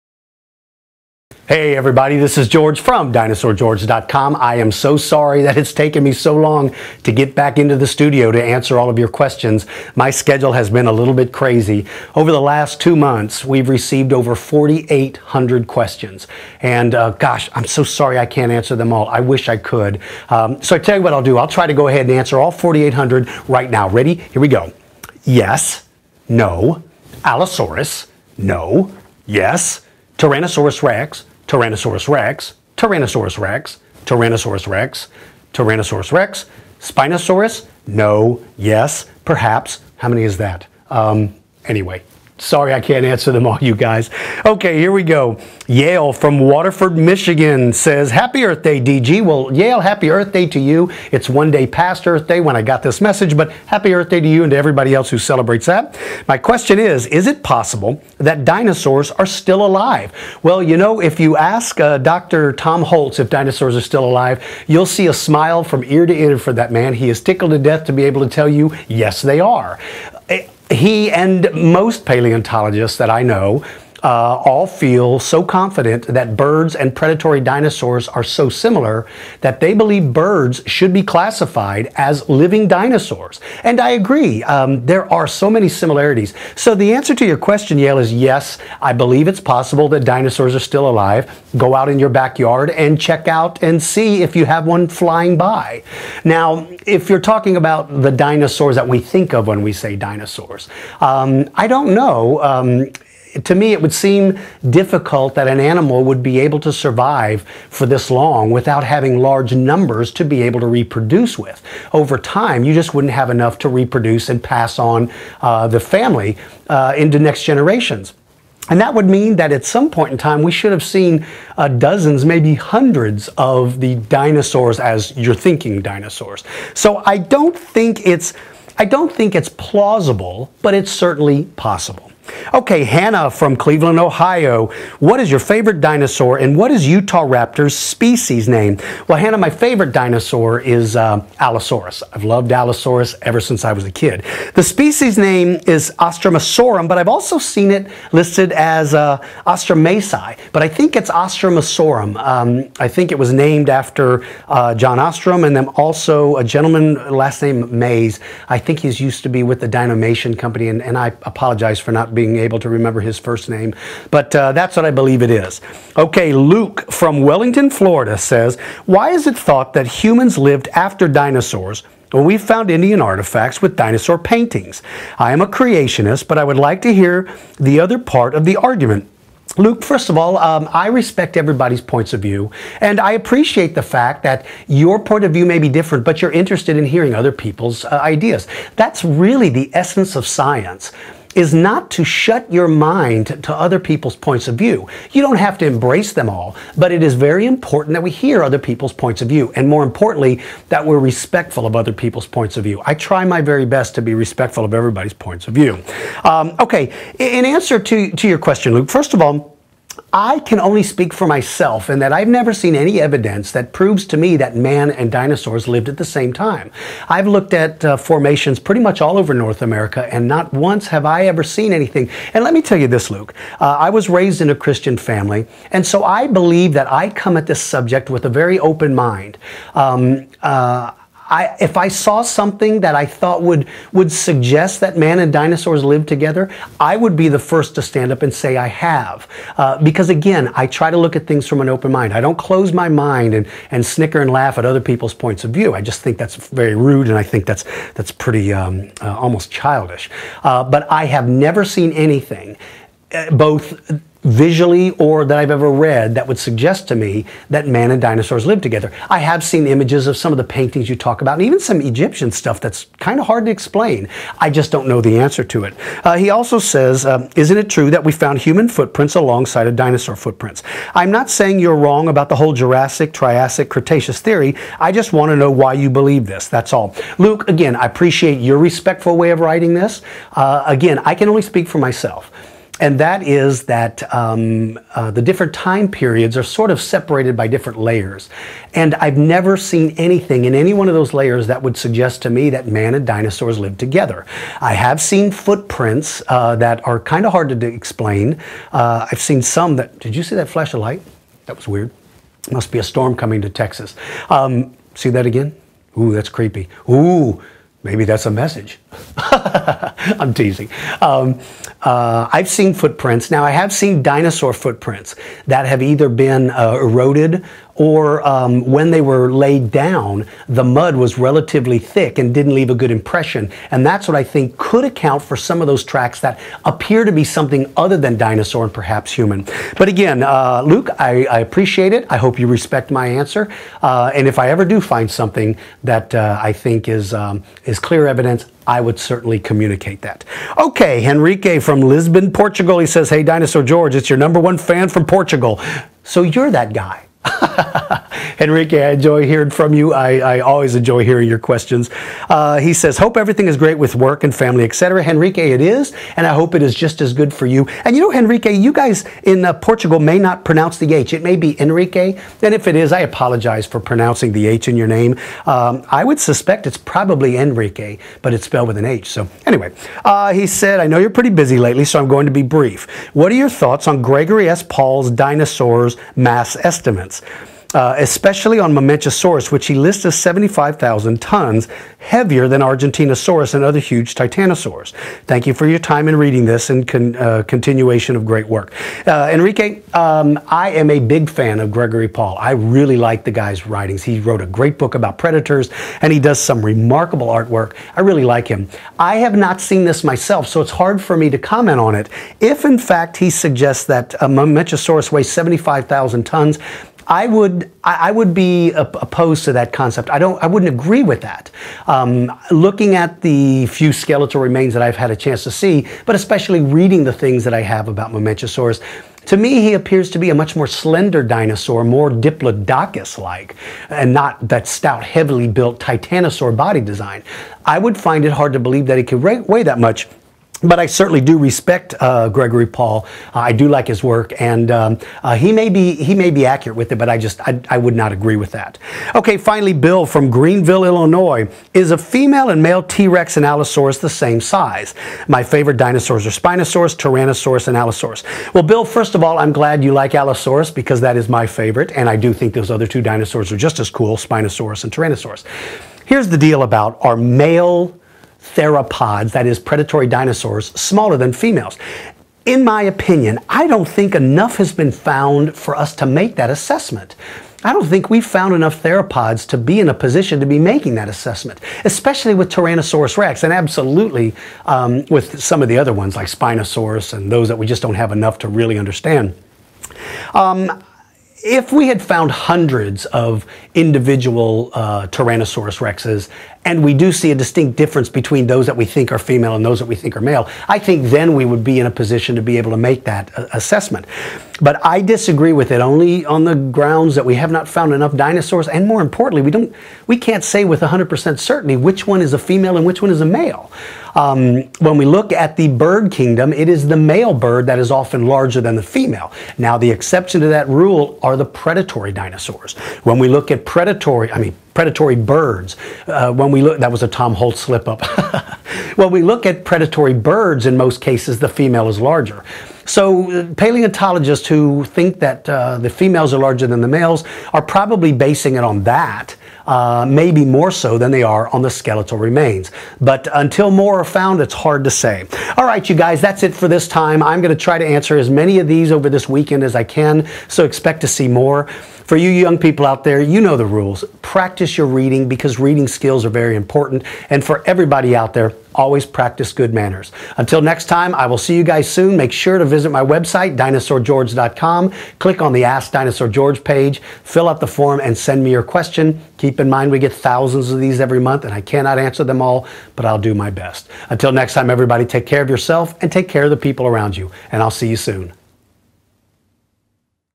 Ask DG #83 - Are Dinosaurs Still Alive?
Dinosaur George answers questions submitted by:  Yale from Waterford, Michigan - Hannah from Cleveland, Ohio - Luke from Wellington, Florida - Henrique from Lisbon, Portugal - Bill from Greenville, IL